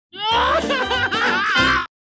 Bai
My reaction to kinemaster shutting down